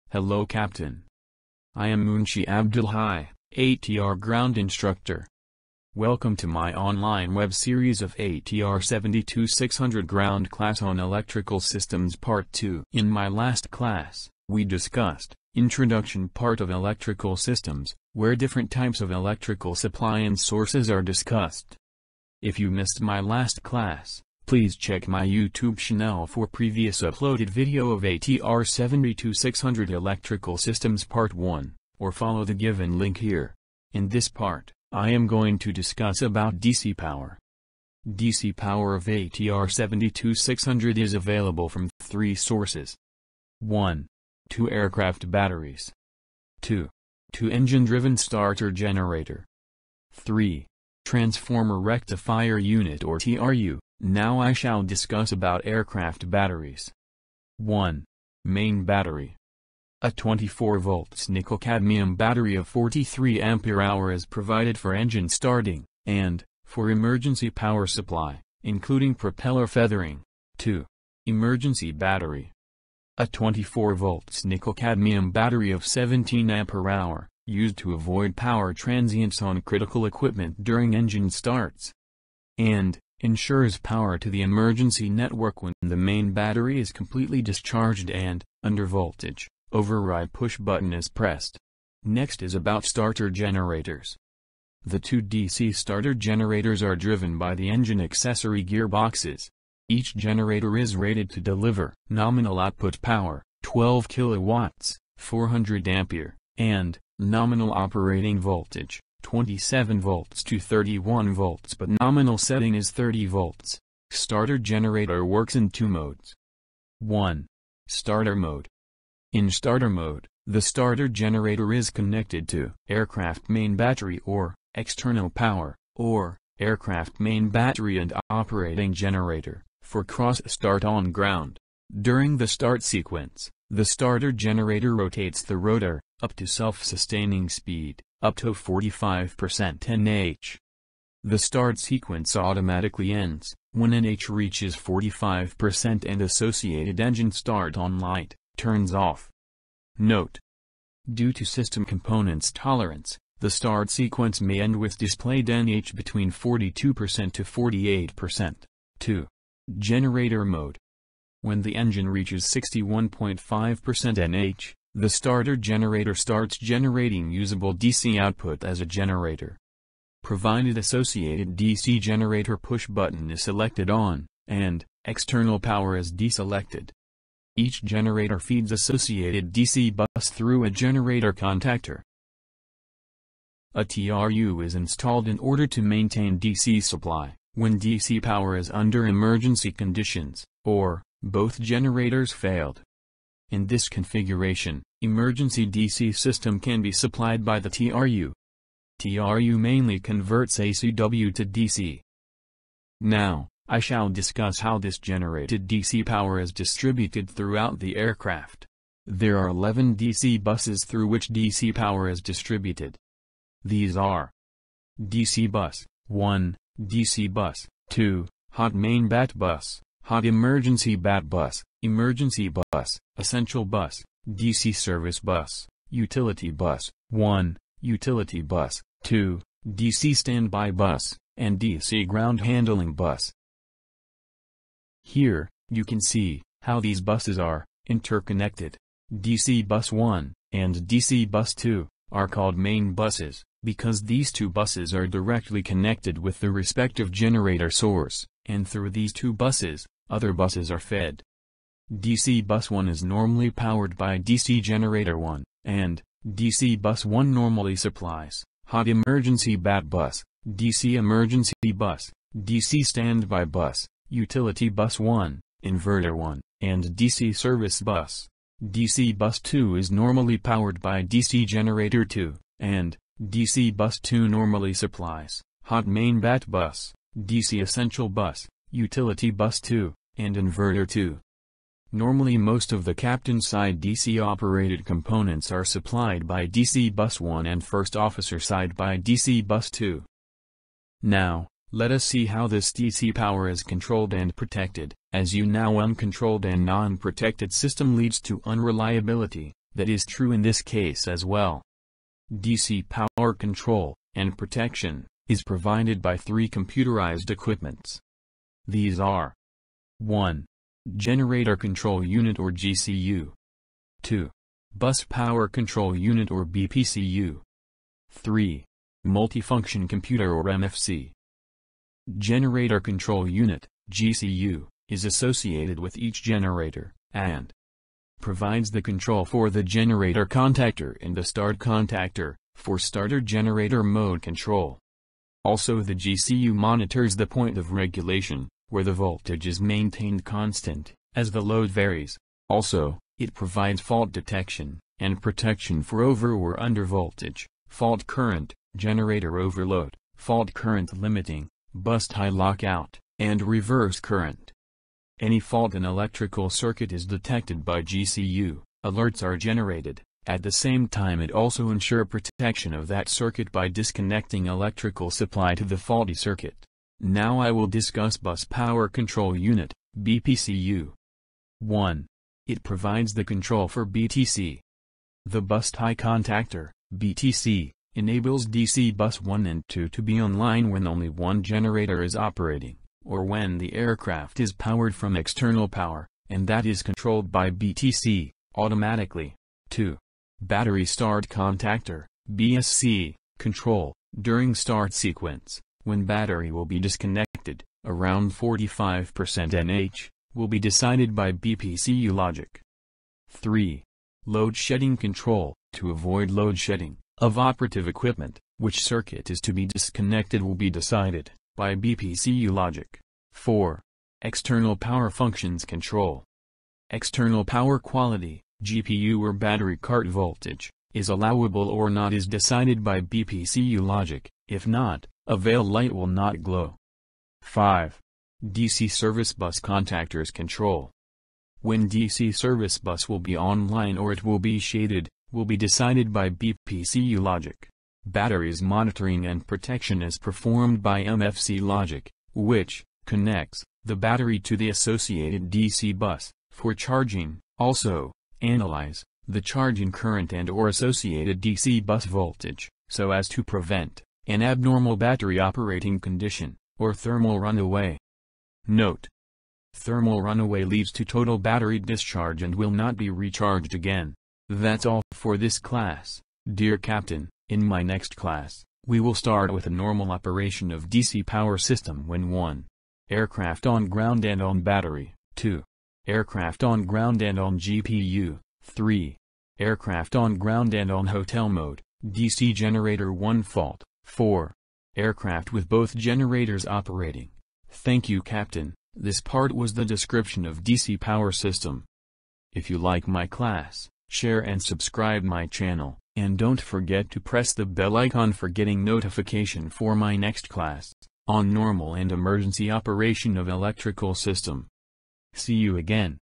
ATR 72-600 Electrical Systems Part - II
Hello Captain!
In this video, I shall give you a description of DC Power of ATR 72-600 Electrical Systems under which, we'll discuss about DC Power distribution and also discuss how to protect and control the same. This is a part of my web series, "ATR 72-600 Ground Class (Technical)"
I am NOT responsible for any damage caused whatsoever due to this video.